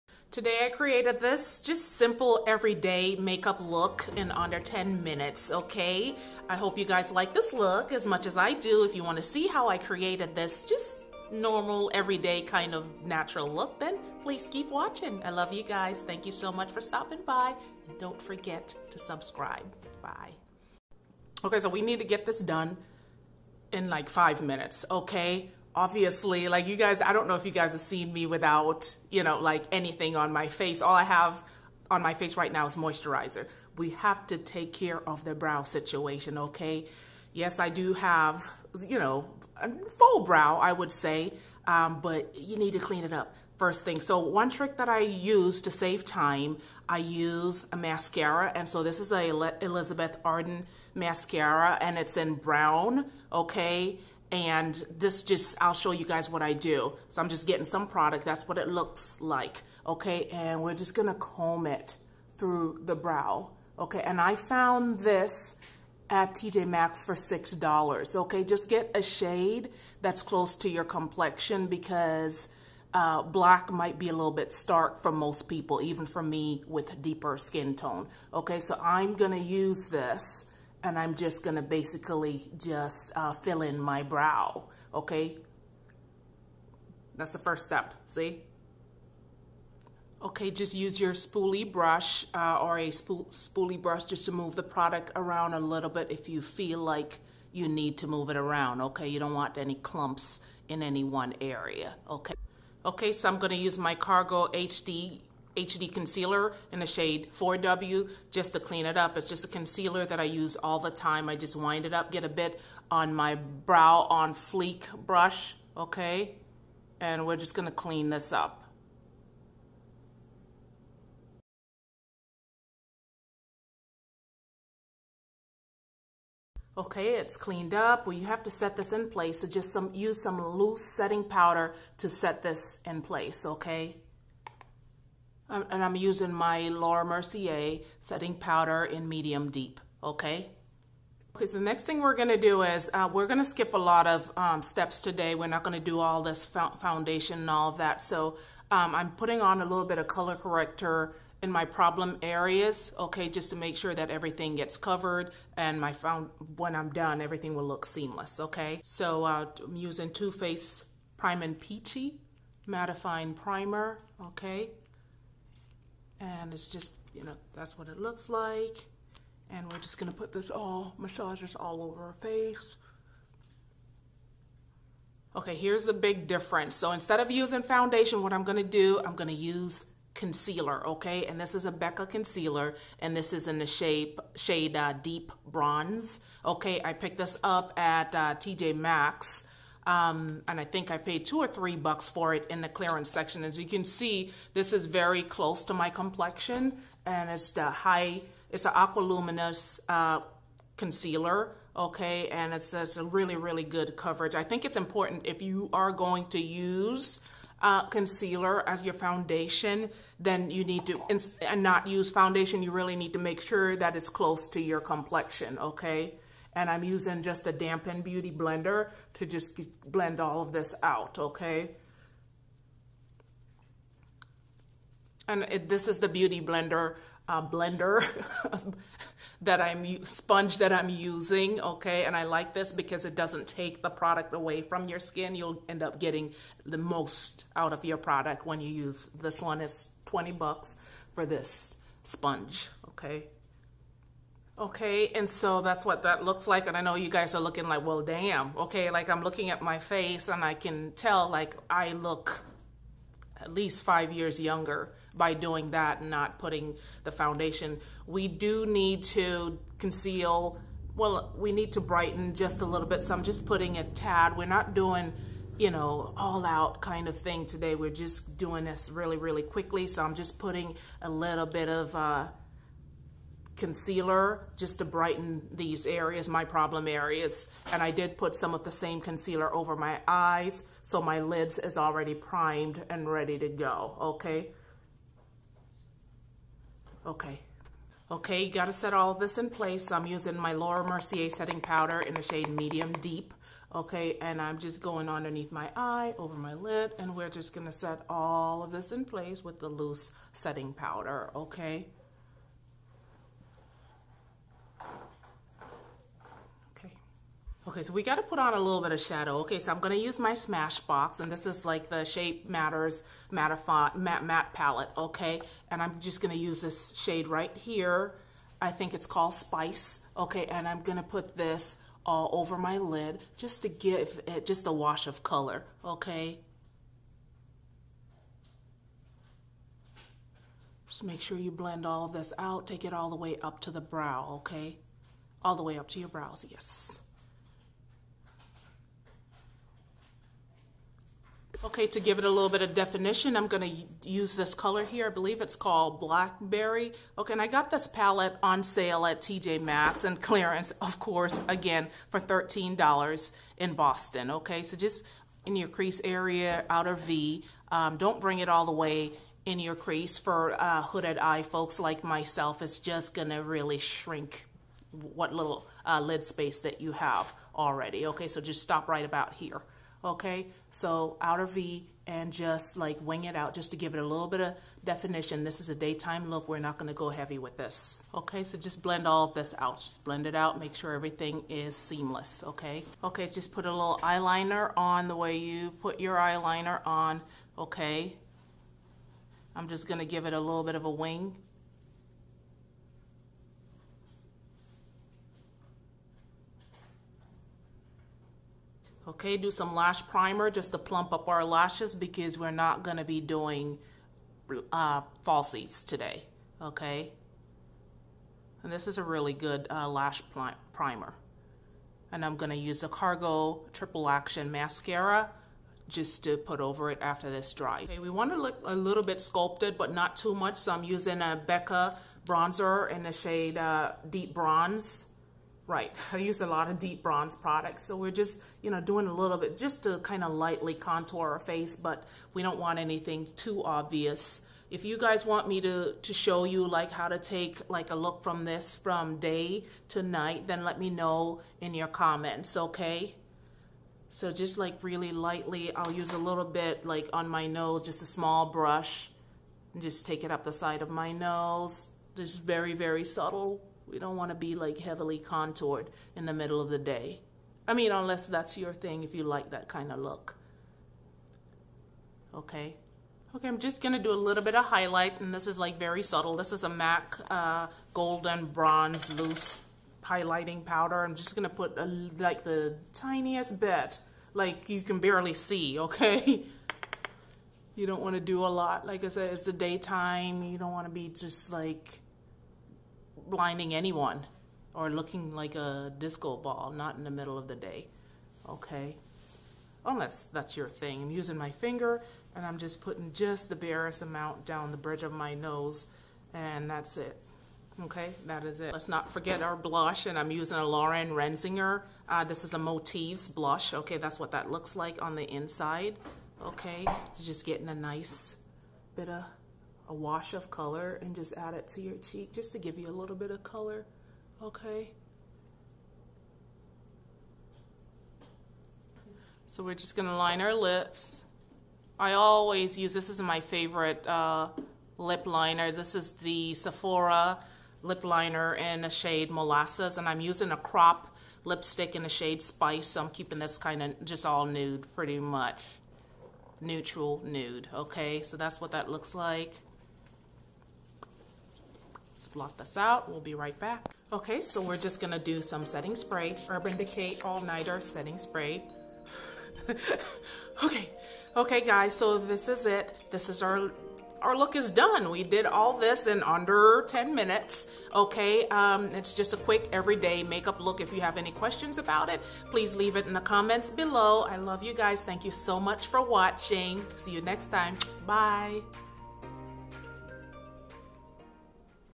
10 minutes makeup tutorial | Everyday makeup look😍
Hi guys,
This is a natural makeup look. Anyone can do this, in under 10 minutes!!! Let me know what you guys think about this look, by leaving a comment in the comments section.
Thank you guys for stopping by.
XOXO
Missi